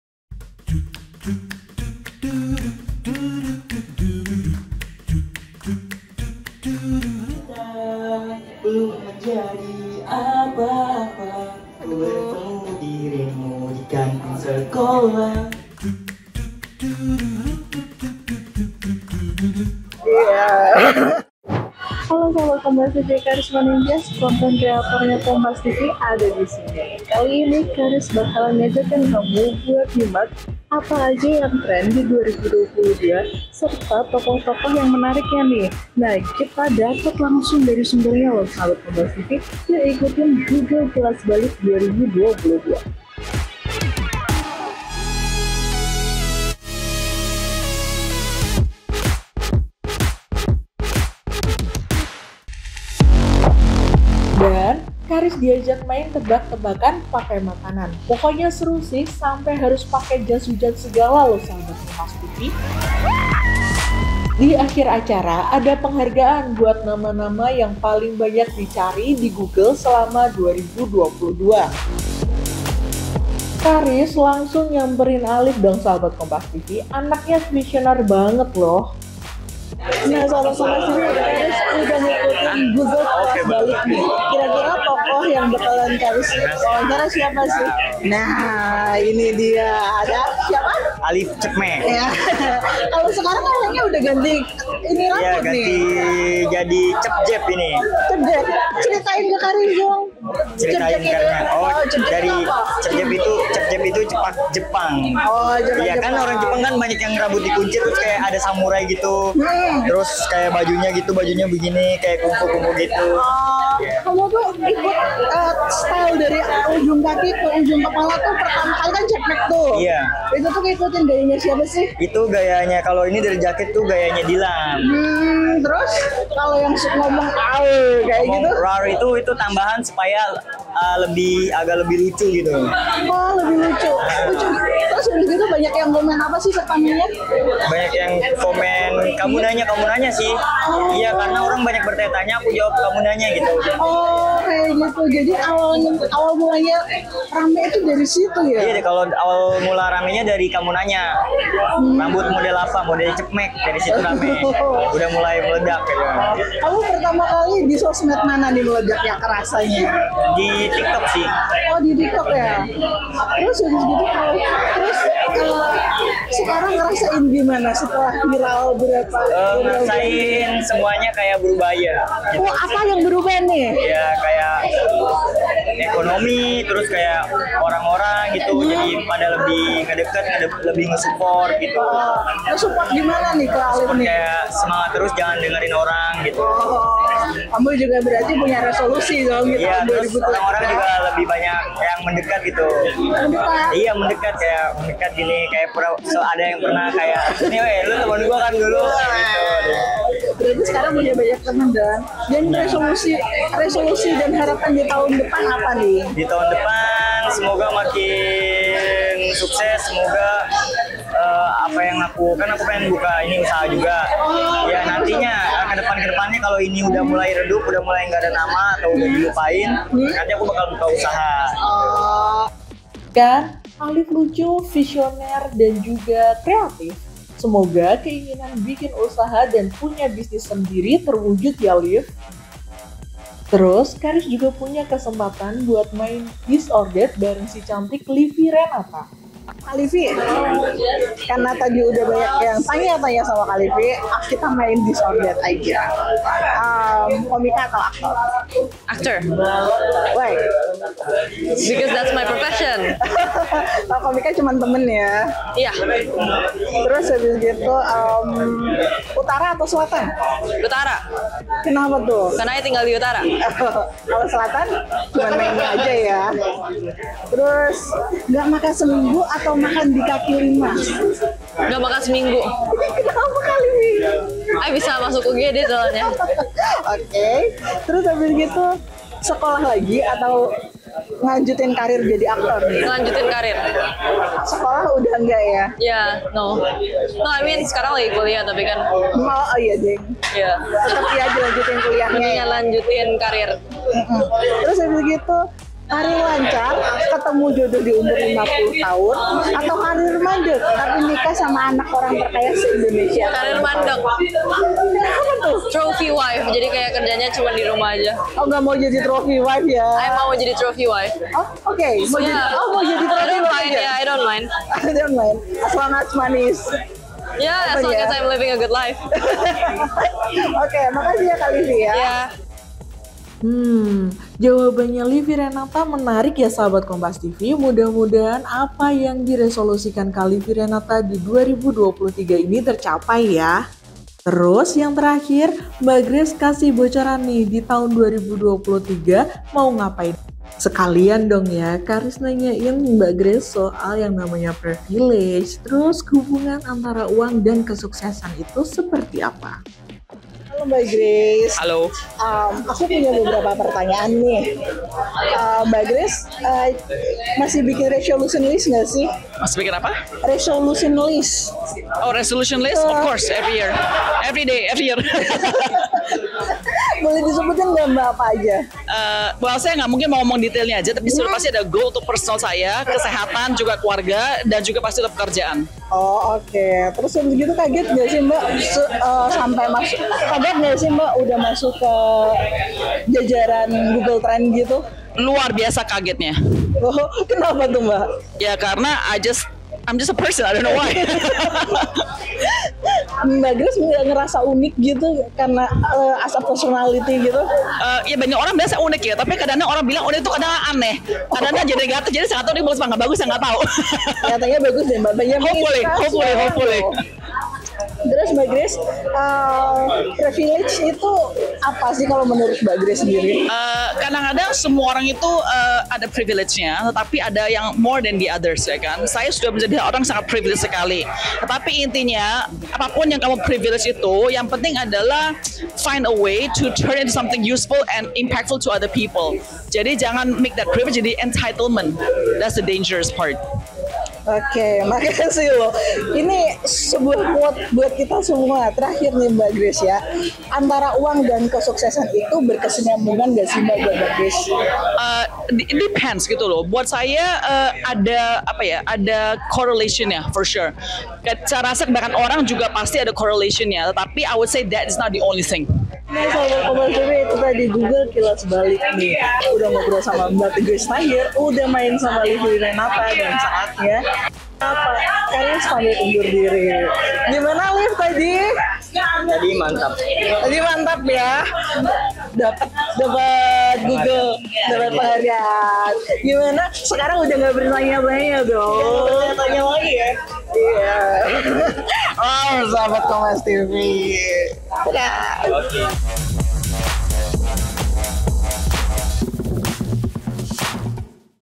Yay! Ketemu Grace Tahir, Alif Cepmek, Sampai Main Game Bareng Livy Renata
JAKARTA, KOMPAS.TV - Diajak main tebak tebakan pakai clue makanan.

pokoknya seru!

Dampe harus pake jas ujan juga.

Di akhir acara, ada penghargaan buat nama-nama yang paling banyak di google selama 2022.

Teruusss, Kharis ngajakin ngobrol mba Gace Tahir dan Alif "Cepmek" yang sekarang rambutnya udah jadi "Cepjep" dan main bareng Livy Renata.